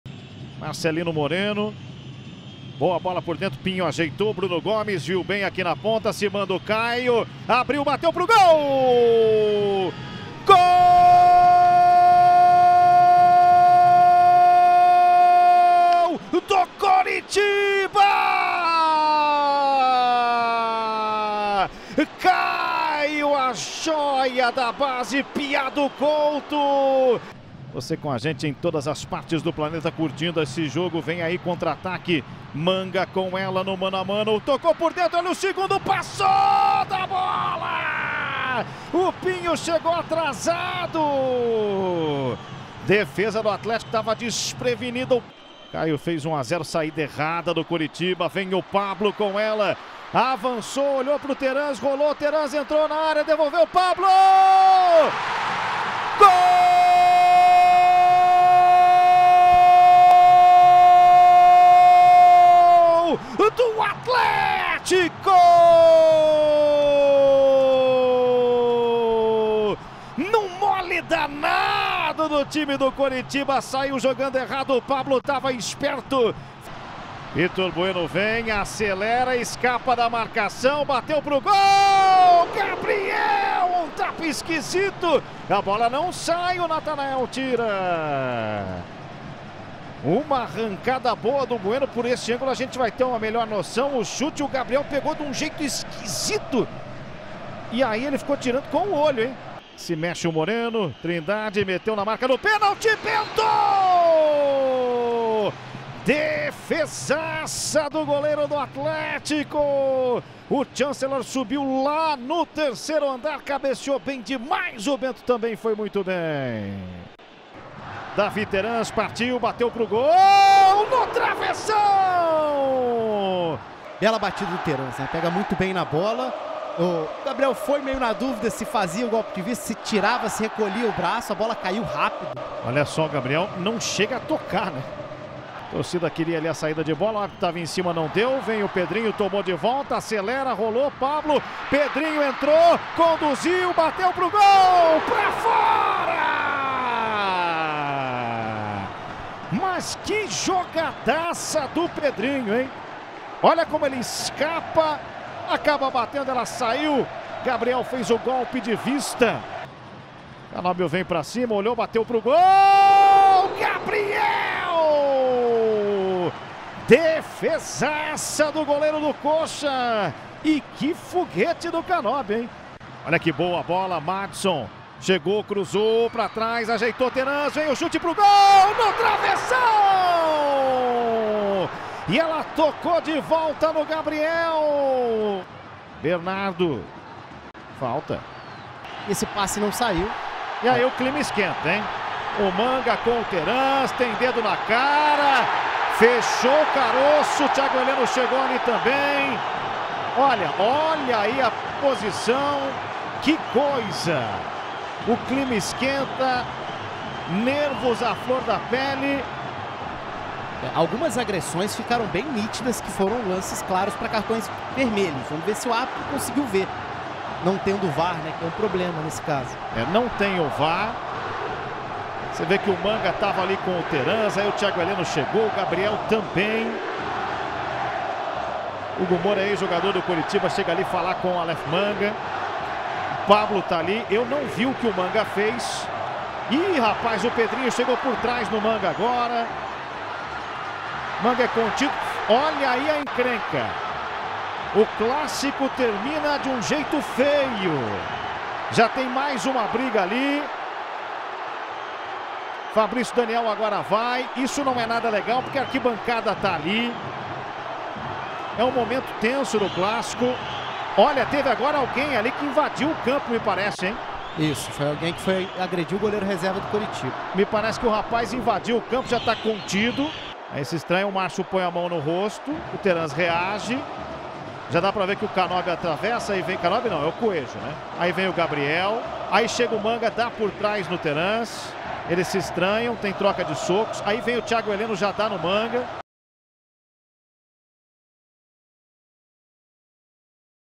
ATHLETICO-PR 1 X 1 CORITIBA | MELHORES MOMENTOS | CAMPEONATO PARANAENSE 2023
Em um jogo marcado por briga generalizada no final, o Athletico Paranaense e Coritiba fizeram um jogo movimentado na Arena da Baixada, mas tudo ficou no empate de 1 a 1, pela sétima rodada da primeira fase do Campeonato Paranaense. Confira os melhores momentos.